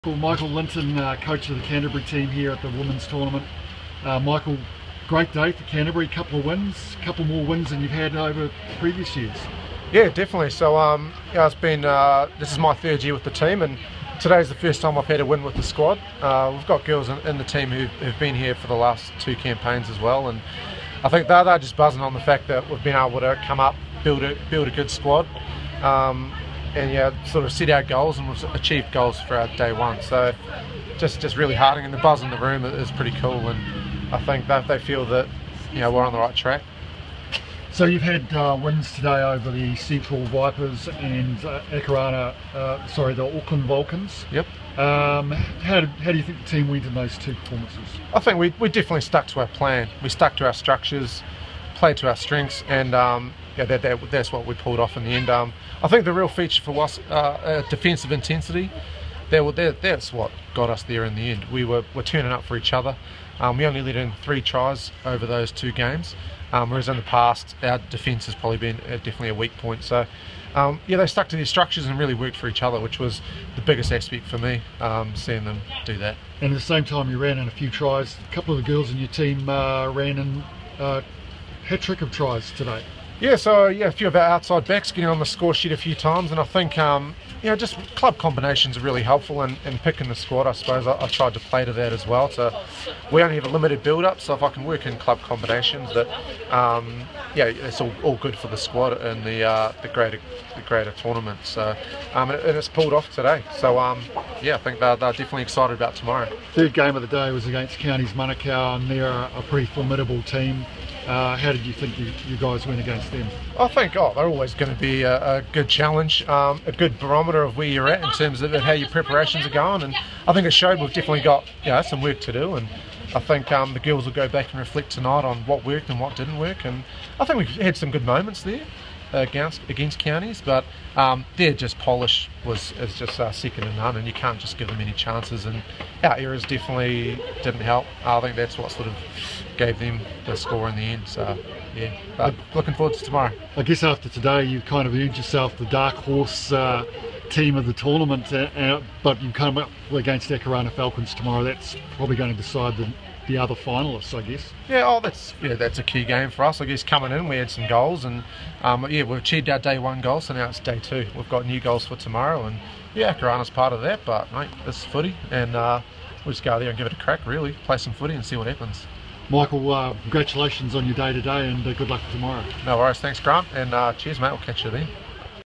NZRL National Women's Tournament - Michael Linton
Canterbury coach Michael Linton reflects on his team's outstanding start to the national women's championship.